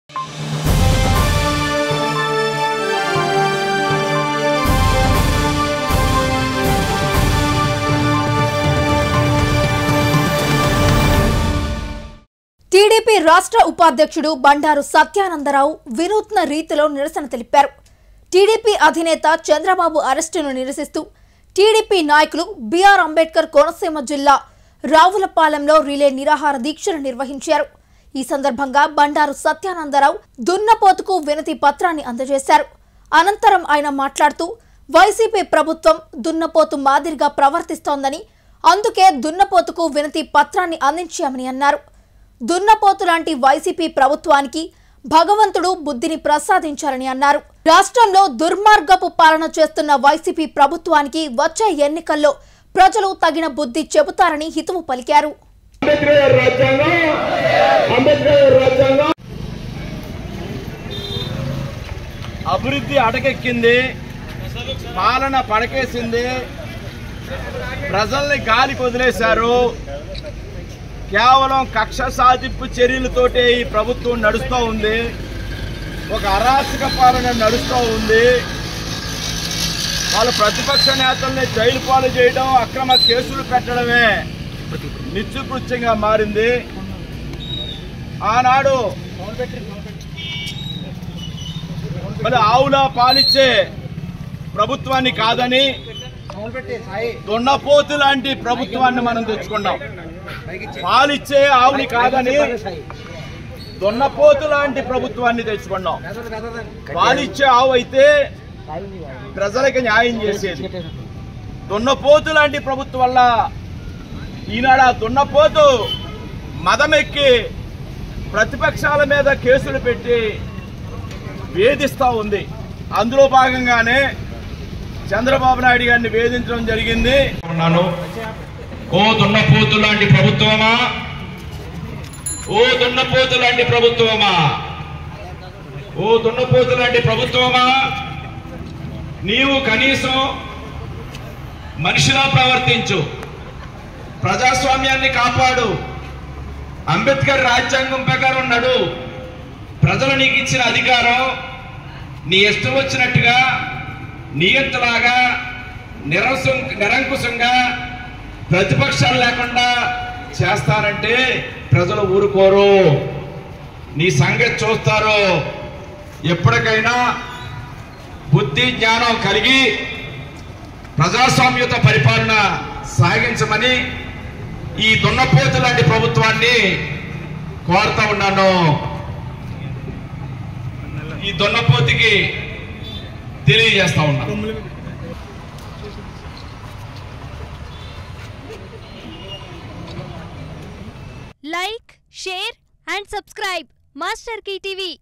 దున్నపోతుకు వినతి పత్రం | TDP State Vice President Bandaru Satyananda Rao | Konaseema District
Category